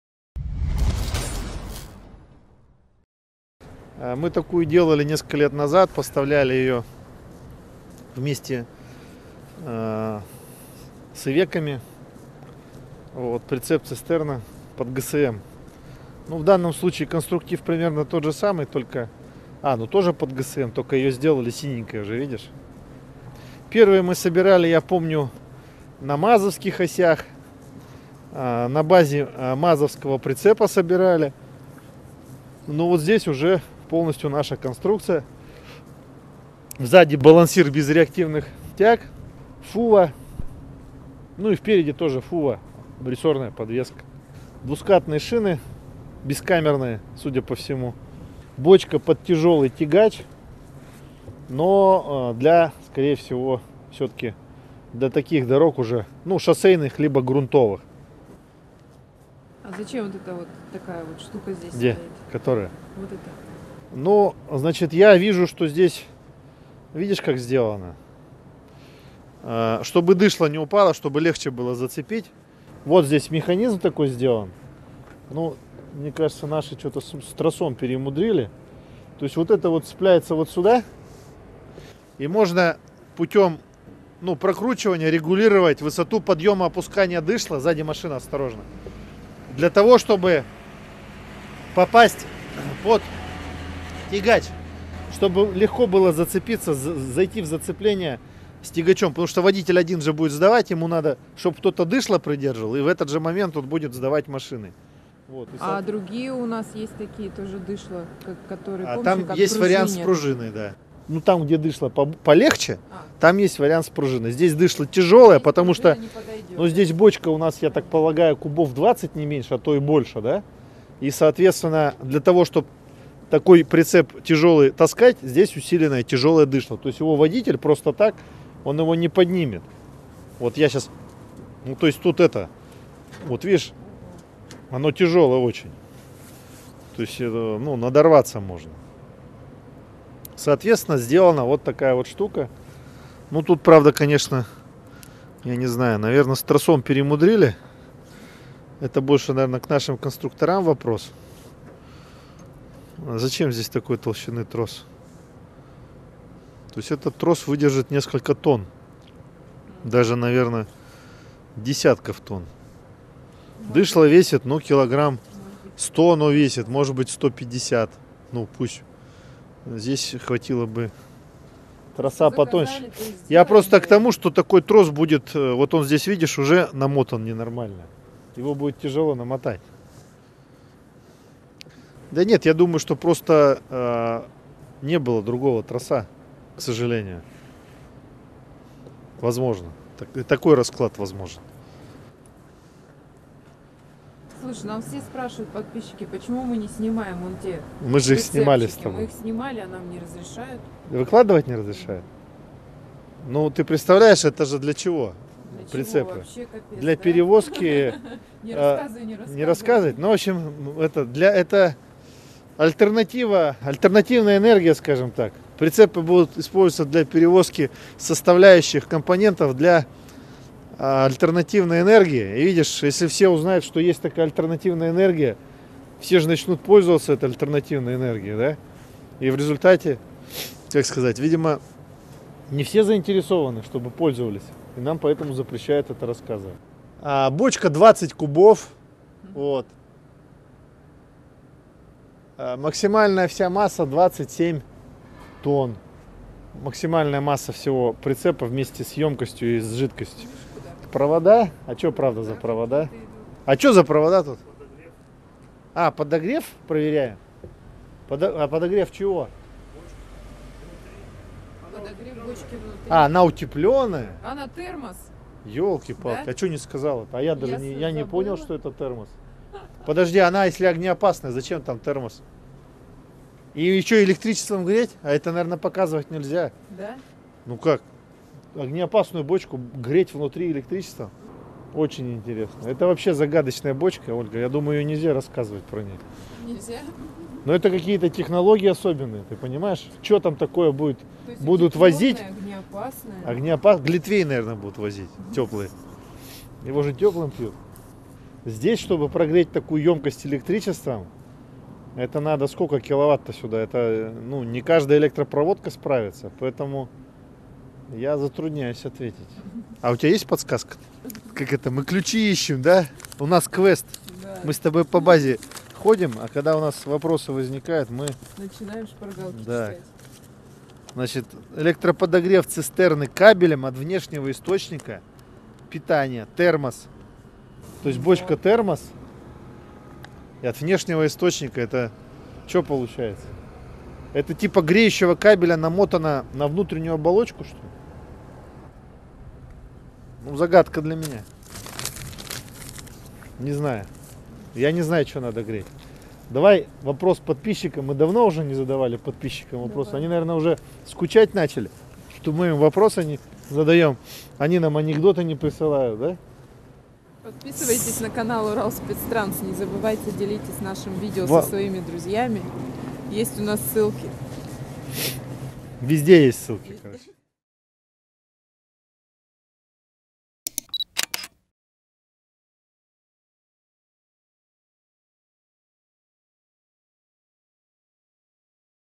Прицеп для ГСМ с электроподогревом ⚡🔥
Прицеп для ГСМ с электроподогревом УралСпецТранс ПЦ20Т-31Д кабелем.

Прицеп-цистерна ПЦ 20Т-31Д УСТ-94651 предназначен для транспортирования и кратковременного хранения светлых нефтепродуктов. Прицеп цистерна ПЦ 20Т-31Д УСТ-94651 оборудован системой подогрева, которая представляет собой саморегулирующий греющий кабель, установленный под термоизолированным корпусом цистерны.

ПЦ 20Т-31Д УСТ 94651, 20 м3, прицеп цистерна, двухскатный, трехосный, рессорная зависимая подвеска, АБС, термос.

Наши контакты:
г. Миасс, пр. Макеева 56.